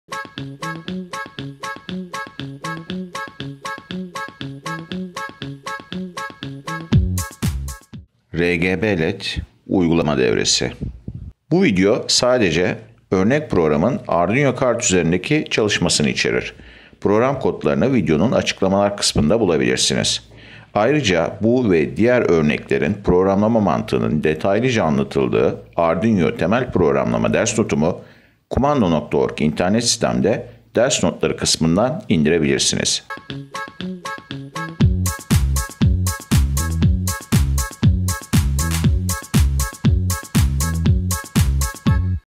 Arduino TEMEL PROGRAMLAMA Örnek – 1/4-1
Çok renkli led - RGB led uygulaması.

Program kodları:

void setup()
pinMode(8, OUTPUT);
pinMode(9, OUTPUT);
pinMode(10, OUTPUT);
void loop()
digitalWrite(8, 1);
digitalWrite(9, 1);
digitalWrite(10, 1);
delay(1000);
digitalWrite(8, 0);
delay(1000);
digitalWrite(8, 1);
digitalWrite(9, 0);
delay(1000);
digitalWrite(9, 1);
digitalWrite(10, 0);
delay(1000);